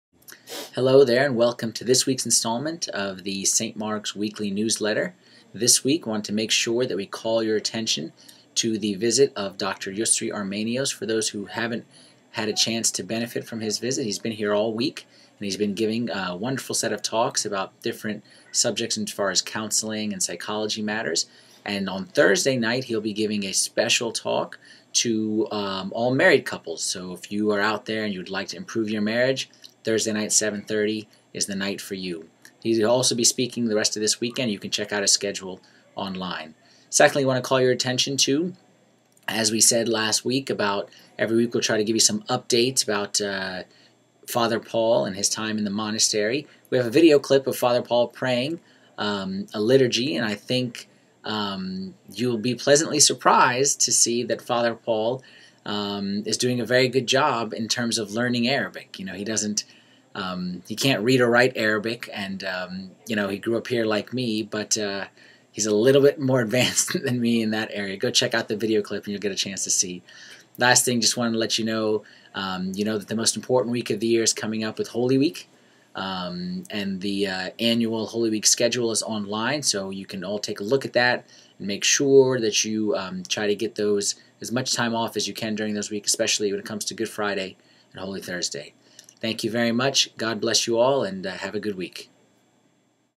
SMWN 3.25.09
Father Anthony highlights this weeks top 3 newsletter items. 1) Couples Seminar this Thursday by reknowned counselor Dr. Armanous. 2) Father Paul learning to pray the Holy liturgy in Arabic, and 3) the Holy Week schedule for 2009. These highlights and all the rest of this weeks activities are detailed in the 3.25.09 newsletter.